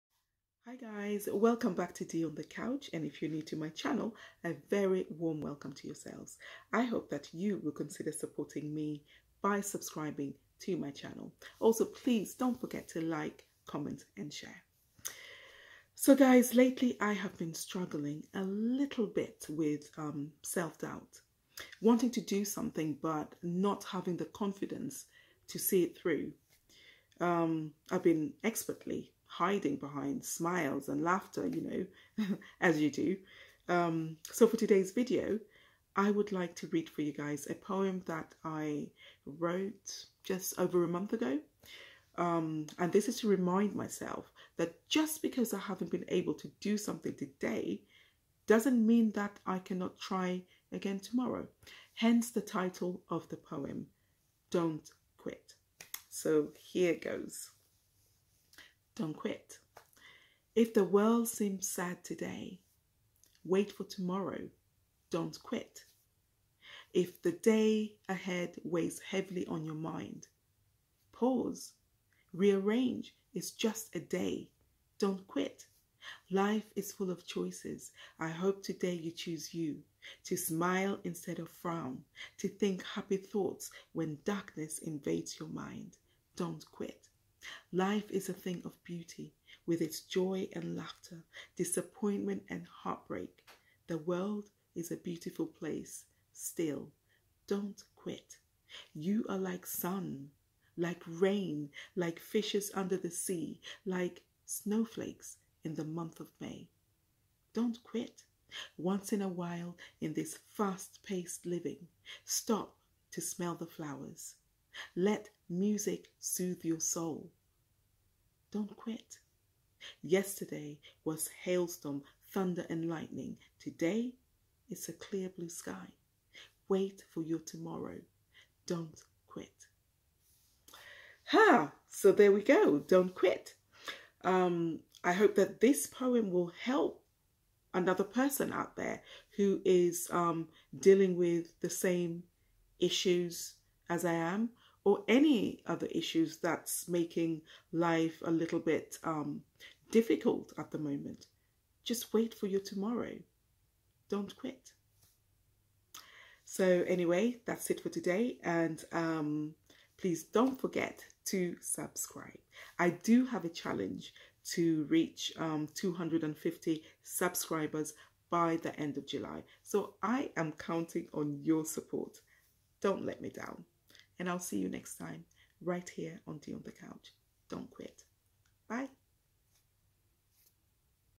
Self-doubt
In this spokenword I talk about selfdoubt and remind myself
We all struggle with self doubt sometimes and need to be encouraged that tomorrow can be a better day to try again.

I hope this will resonate with someone and that it will encourage them to wait for their tomorrow.